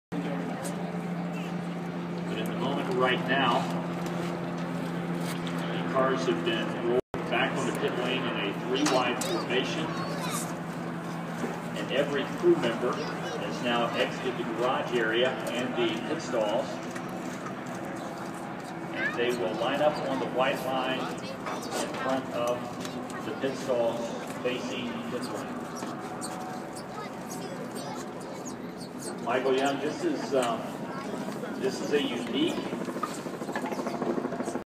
Somber Crowd at Las Vegas Motor Speedway
Sadness after the insane Indy car crash.